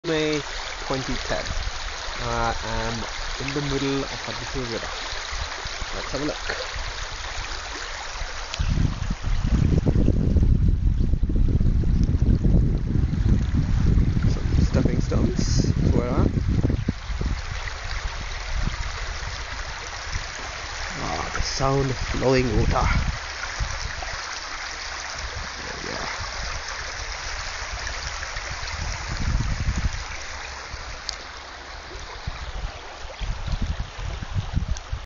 On the stepping stones by the river
Standing on some stepping stones and enjoying the sound of running water